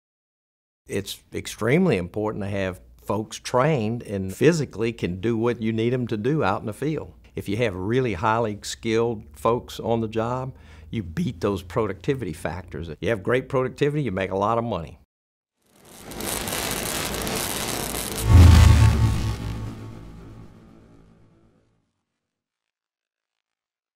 NCCER Training - Contractor Perspective
Andy Dupuy, President/CEO of Brown & Root, discusses the importance of training in today's construction industry.